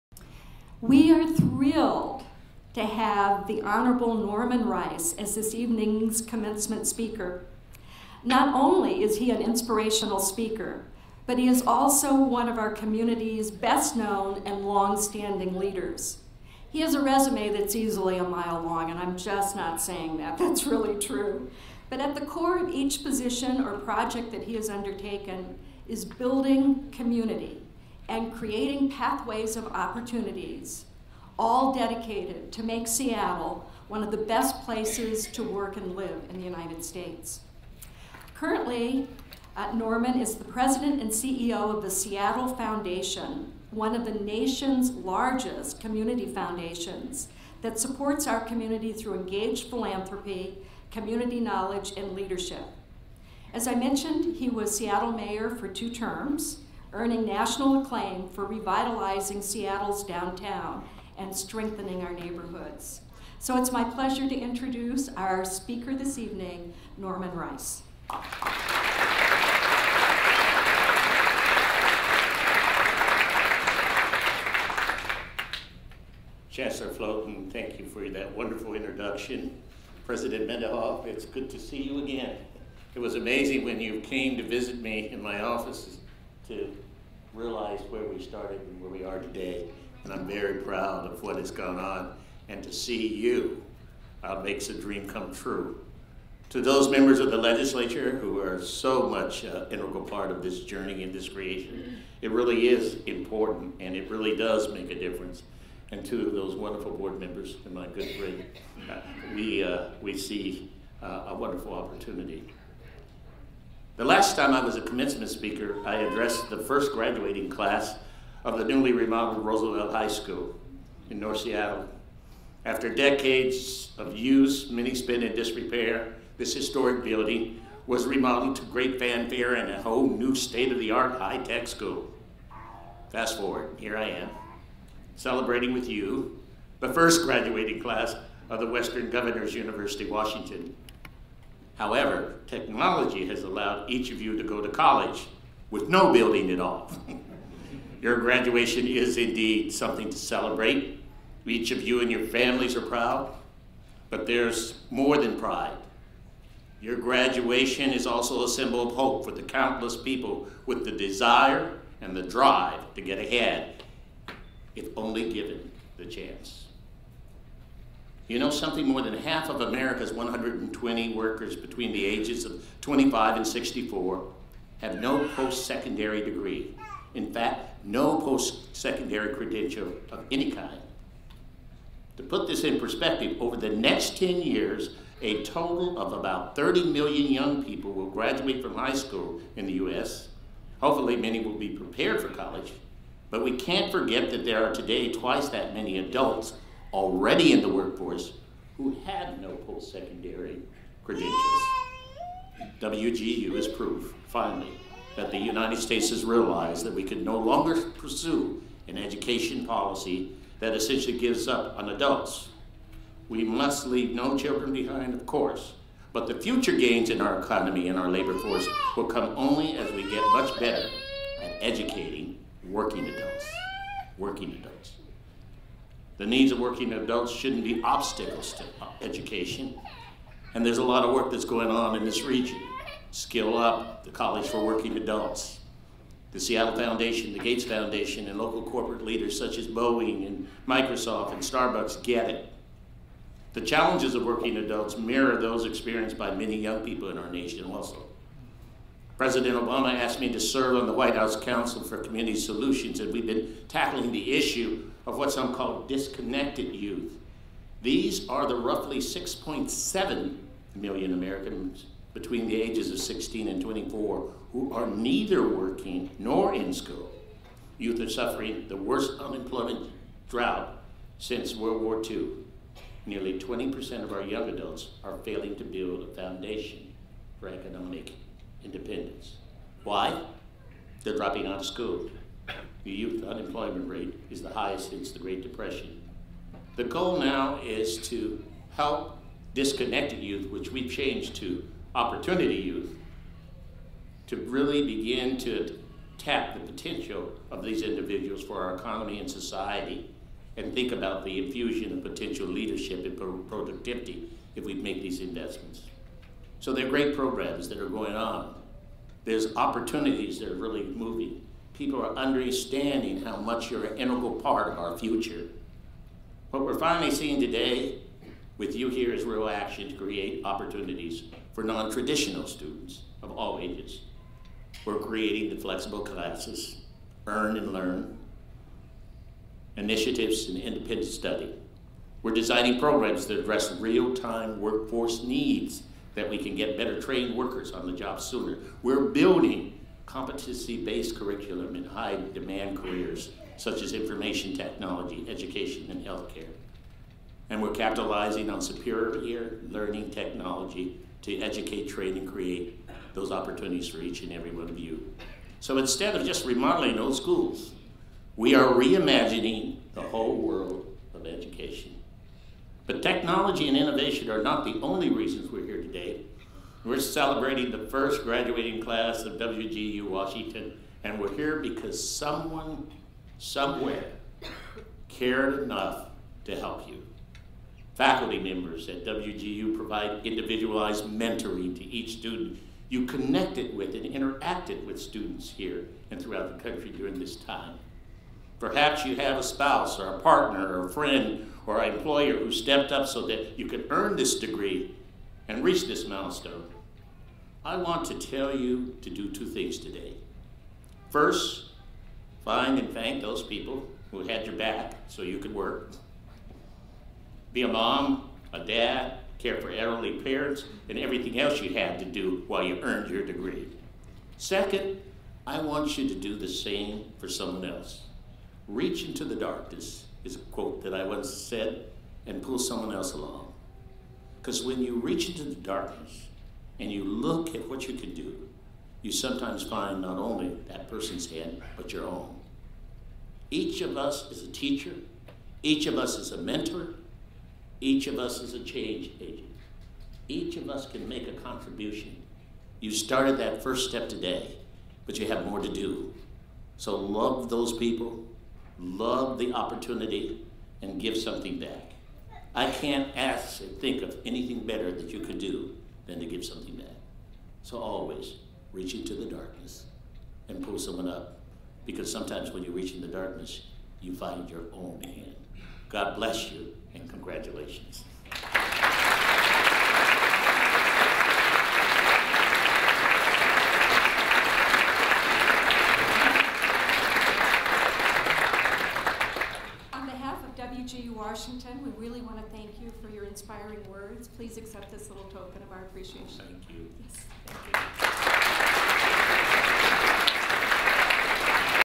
Former Seattle Mayor Norman Rice, online university commencement speaker
- Former Seattle Mayor Norman B. Rice, President and CEO of The Seattle Foundation, congratulates Washington online university graduates at WGU Washington's first-ever commencement ceremony.